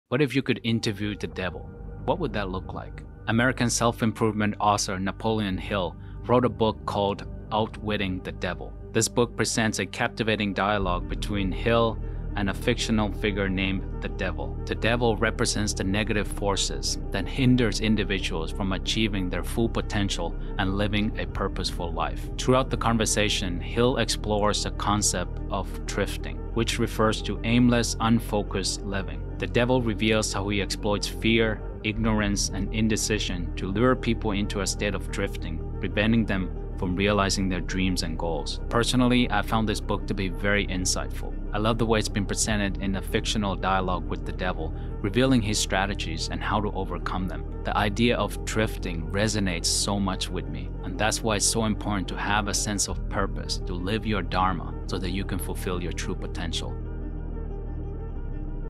Outwitting The Devil | Napoleon Hill Inspirational Book for Success & Purposeful Life
"Outwitting the Devil" by Napoleon Hill is a remarkable and thought-provoking book that delves into a fascinating conversation between the author and an imaginary figure representing the devil. Through their dialogue, Hill explores the strategies employed by the devil to control human lives and hinder personal growth. The book offers insightful principles and strategies to overcome these negative influences and regain control over one's life. An entertaining and enlightening book, I highly recommend